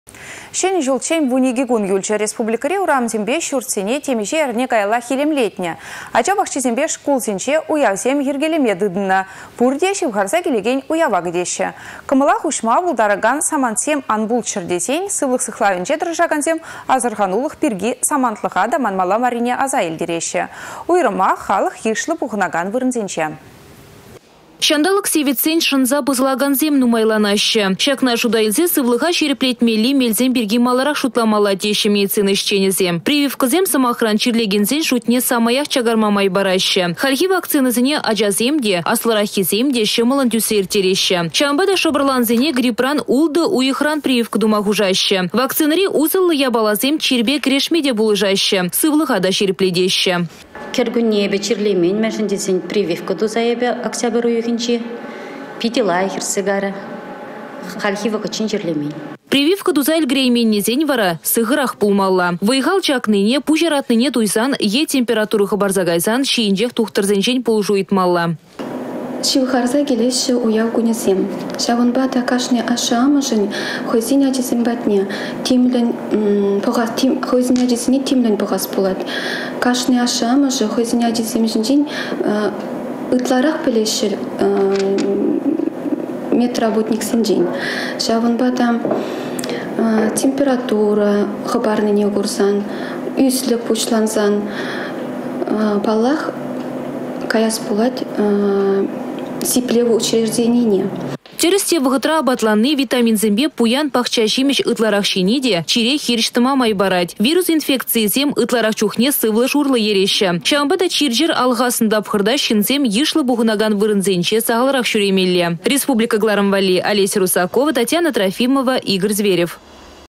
Асăрханулăх пирки манмалла мар
Çĕнĕ çулччен 12 кун юлчĕ. Республикăри урамсемпе çуртсене темиçе эрне каяллах илемлетнĕ. Ача пахчисемпе шкулсенче уявсем йĕркелеме тытăннă та ĕнтĕ. Пурте çывхарса килекен уява кĕтеççĕ. Кăмăла хуçма пултаракан самантсем ан пулччăр тесен сывлăх сыхлавĕнче тăрăшакансем асăрханулăх пирки самантлăха та манмалла маррине аса илтереççĕ. Уйрăмах халăх йышлă пухăнакан вырăнсенче.